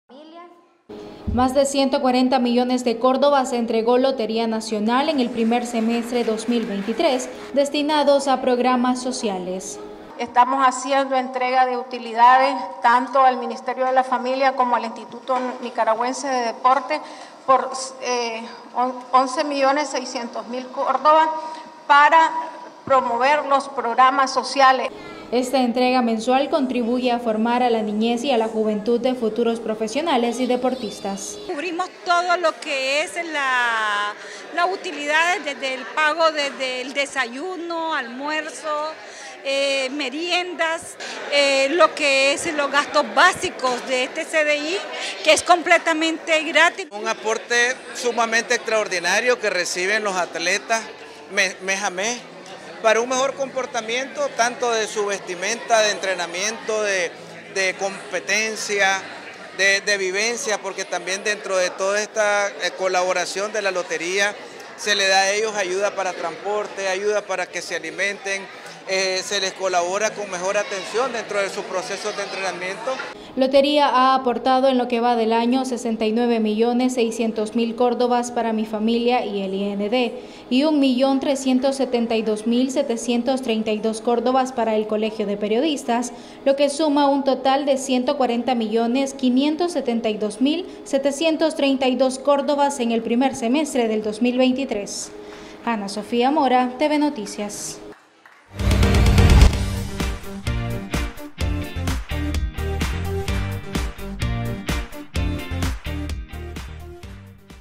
Lotería Nacional entrega más de C$11 millones al IND y Mifam
Lotería Nacional entrega utilidades del mes de junio al Instituto Nacional de Deportes (IND) y al Ministerio de Familia (Mifam) para programas sociales.